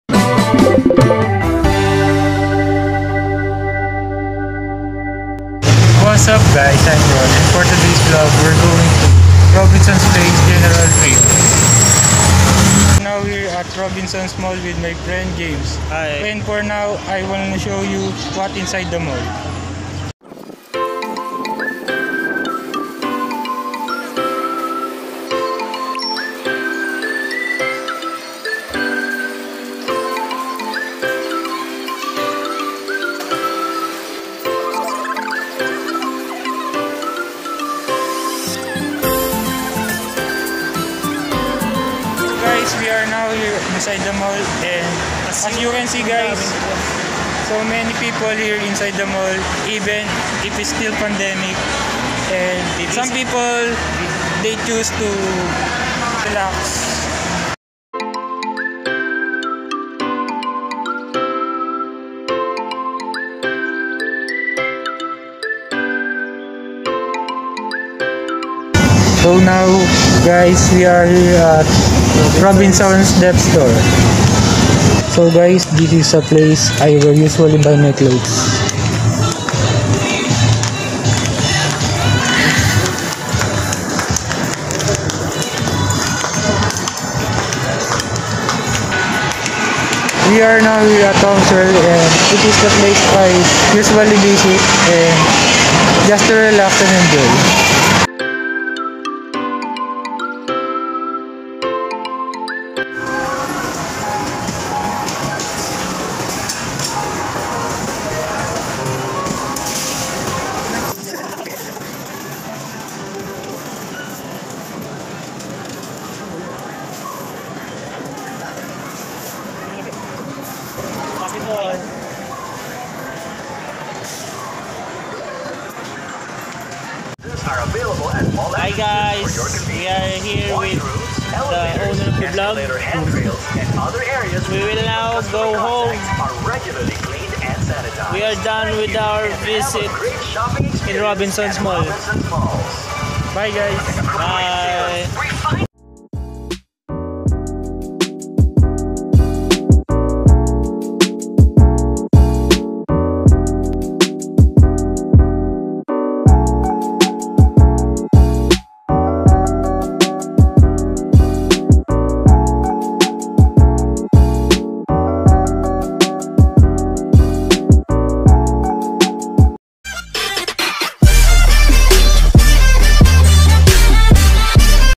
Robinson's Place Gentrias Travel vlog #01
My 1st vlog for school purposes.
Ronald F. Diaz
Cavite State University Ccat Campus